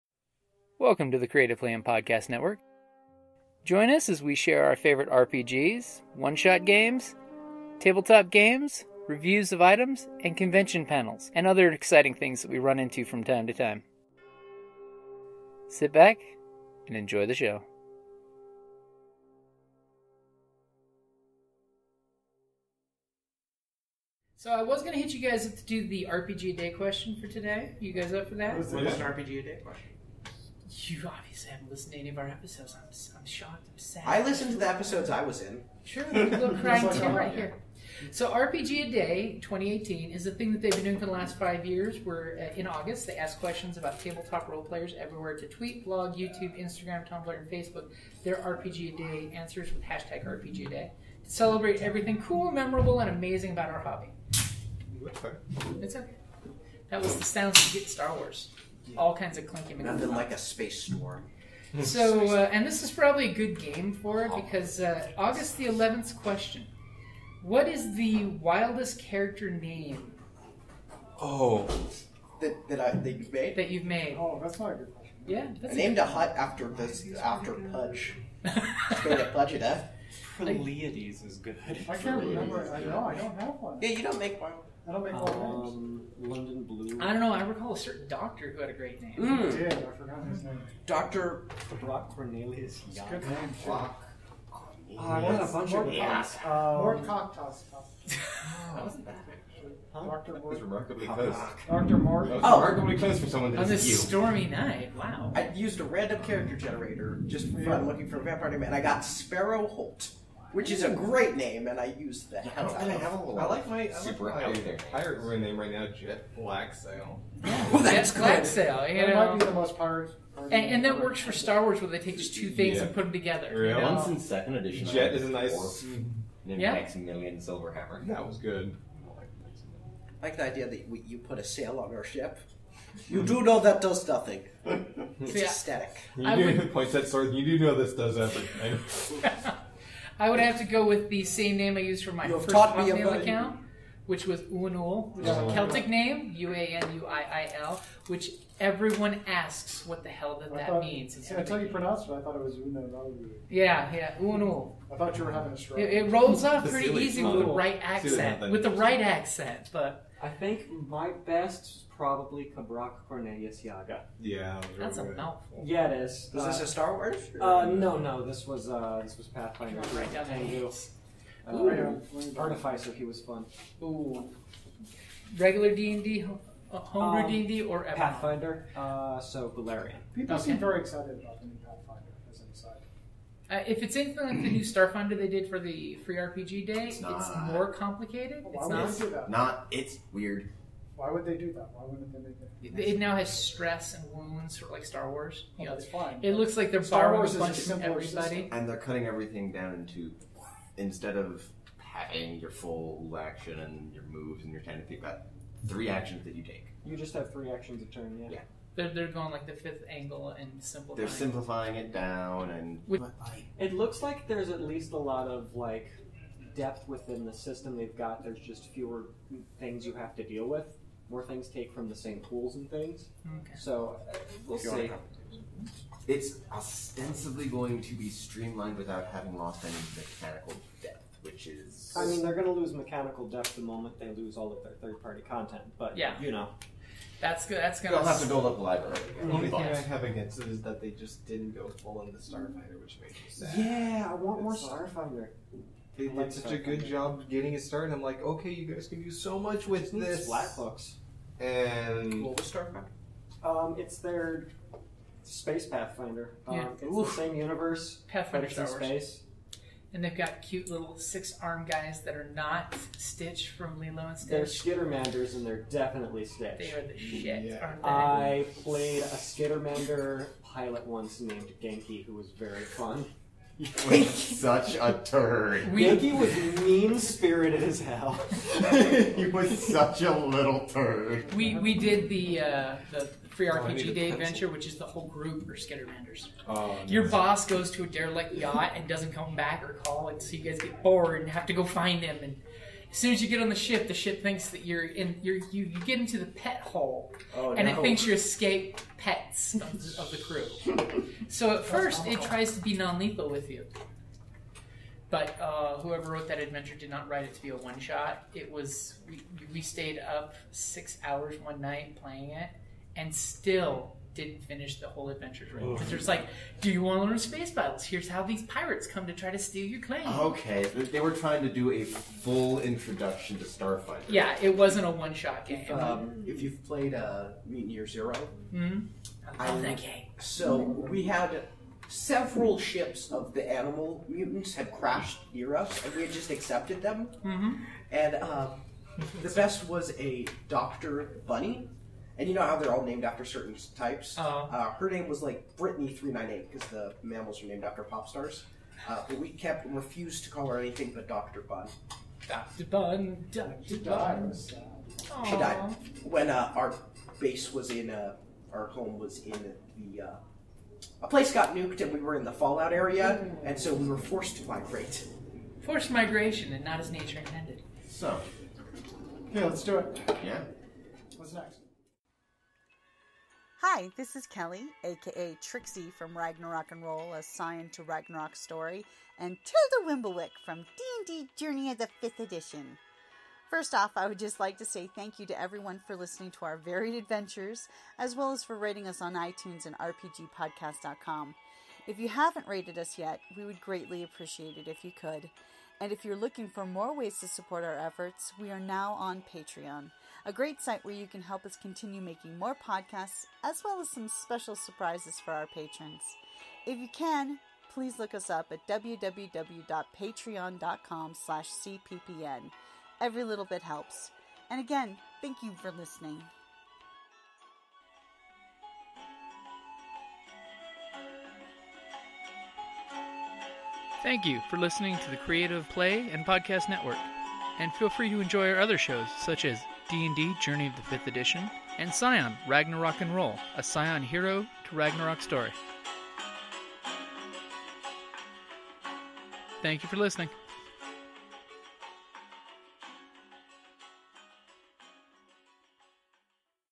RPGaDay2018 August 11th Wildest character name
We ask the new 'Mercs, a Galaxy of Destruction' our August 11th question: Wildest character name?
Join Eli, AJ, Ian, Gary, Xander and me for todays question.
 Dr Bedlam was Garys name.

and RPGaDay2016 episodes here!